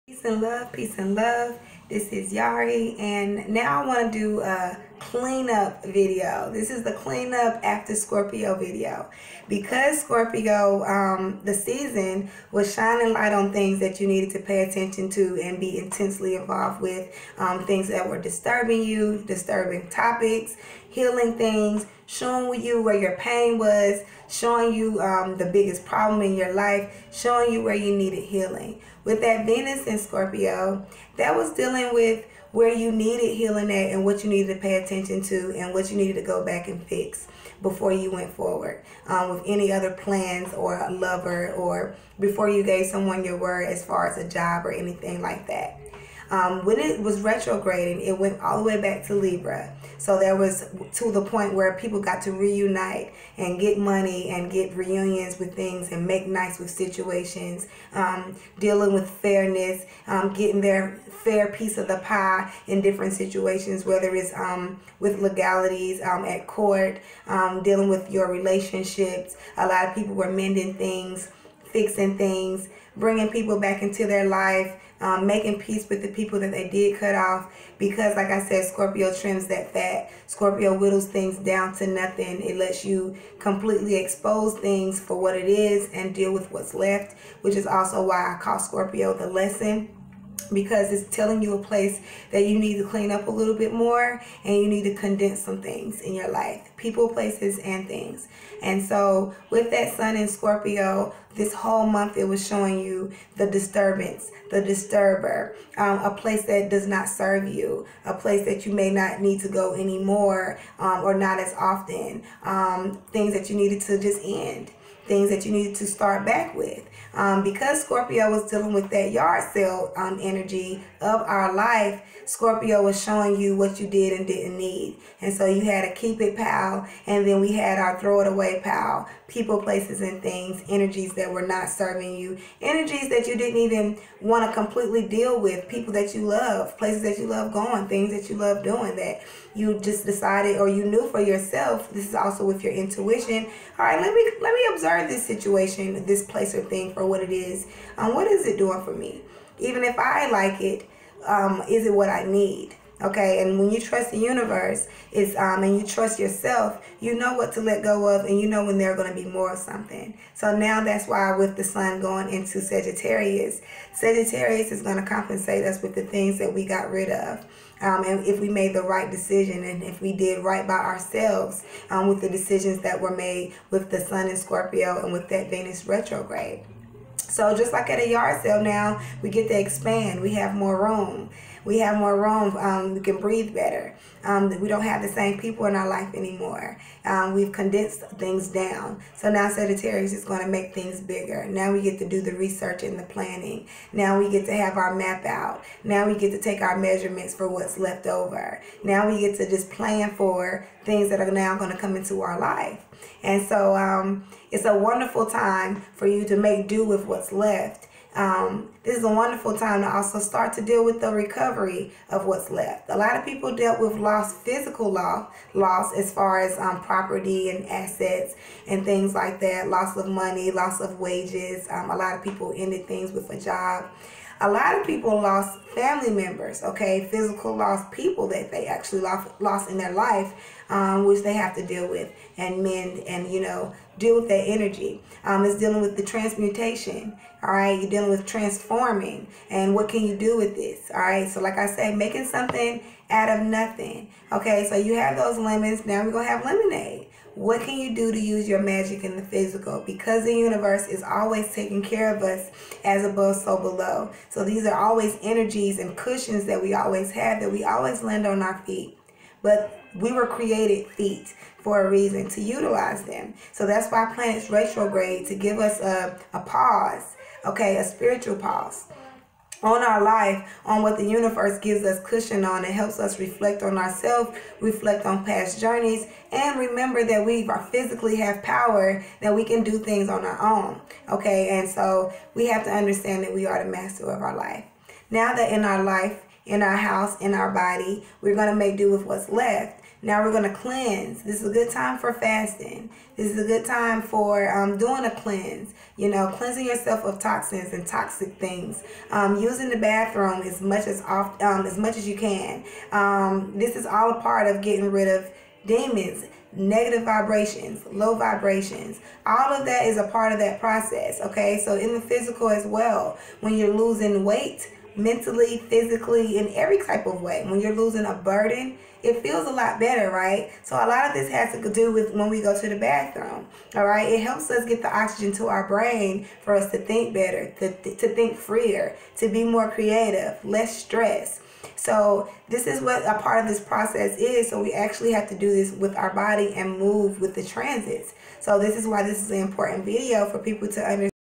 After Scorpio Clean up! Cleanse and Fast in all ways!
This is a time that you should truly be clearing all of the negative energy and recuperate from the Venus Retrograde and Sun in Scorpio.